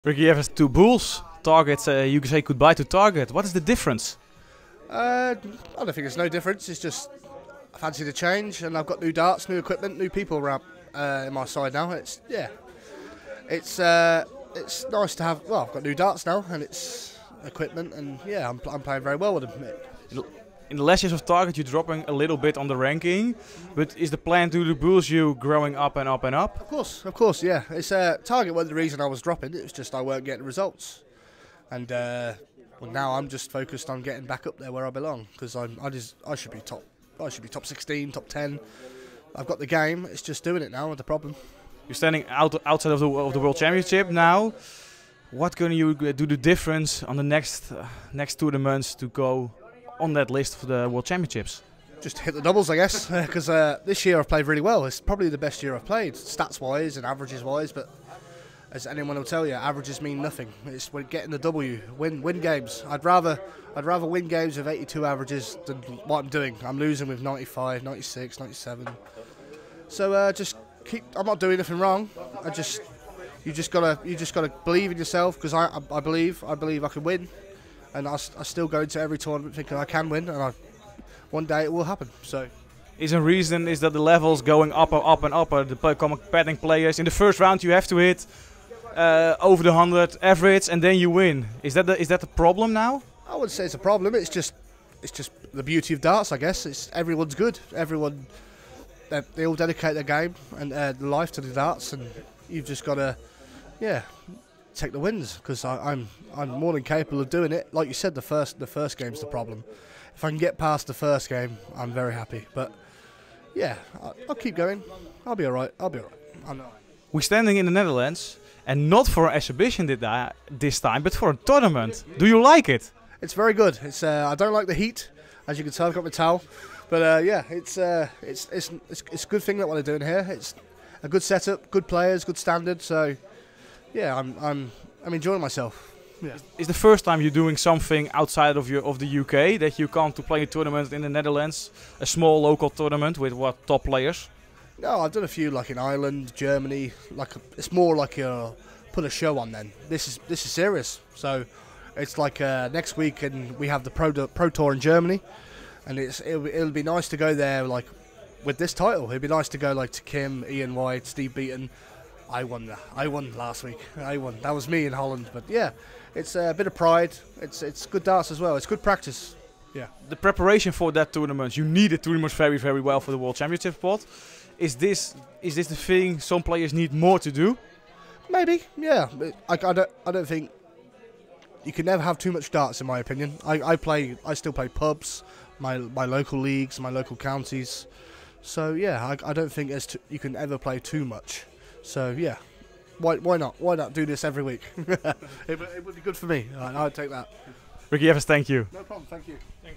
Ricky Evans about switching to Bulls NL: 'I am now focused on back up where I belong'
Ricky Evans made his debut at the sixth edition of the Zwaantje Masters in Eindhoven last weekend. The Englishman has had a challenging year so far with his new darts after switching from Target Darts to Bulls NL earlier this year. In terms of statistics, it's one of Evans' better years, but the results have been lacking. What could be the reason for this?

Darts Actueel-reporter Mike Hooiveld spoke to 'Rapid' about his switch to Bulls NL, 2023 so far, the reasons behind the lack of results and his experiences at the Zwaantje Masters.